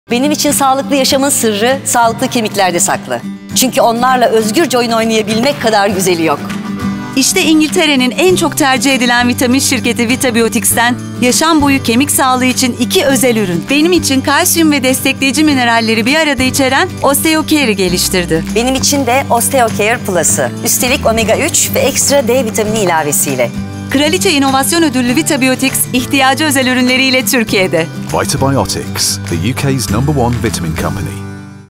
Yaşam Boyu Kemik Sağlığı | Osteocare®
Osteocare® Original; vitamin D3, kalsiyum, bakır, çinko, bor ve selenyum gibi temel elementleri besin referans değerinde içermenin yanı sıra magnezyum gibi kofaktörleri de içerir.
Osteocare® Plus, içerdiği Kalsiyum ve kofaktörlerine ek olarak Omega-3 ve Soya İzoflavonları içermektedir. Bu nedenle Osteocare® Plus, ileri dönem kemik sağlığını desteklemek için dizayn edilmiştir.